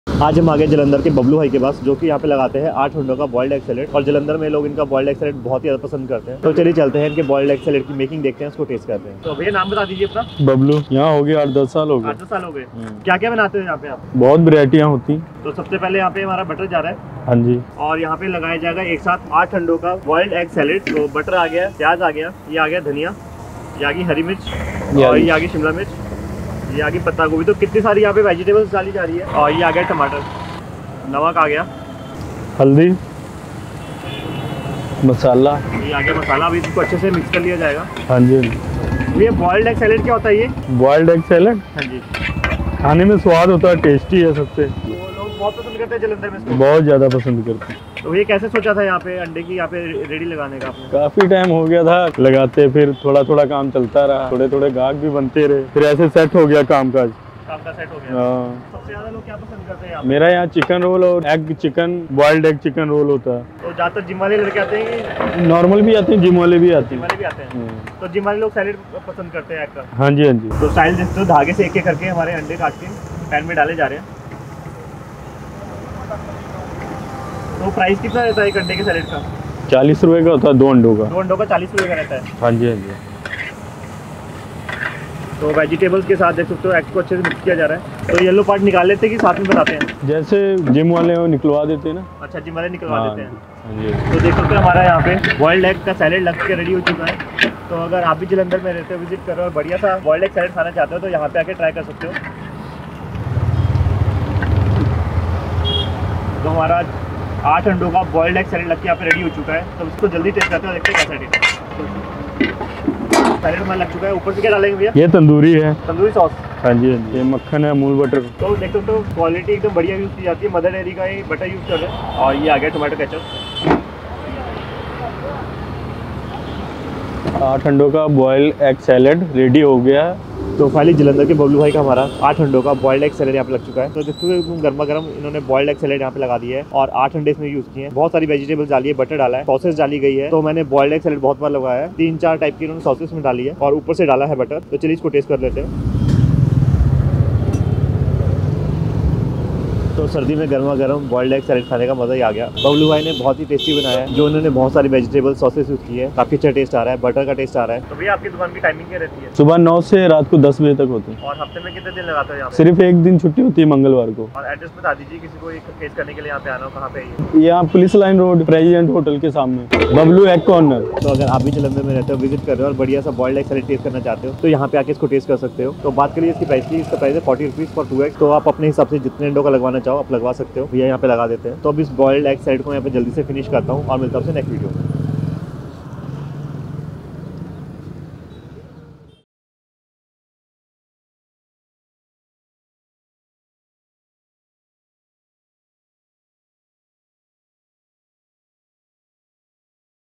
Famous Bablu Bhai Ka Boiled Eggs Salad Making In Jalandhar Rs. 40/- Only l Punjab Street Food
Address: 27, Police Line Rd, opposite Big bazaar, Jawahar Nagar, Jalandhar, Punjab 144001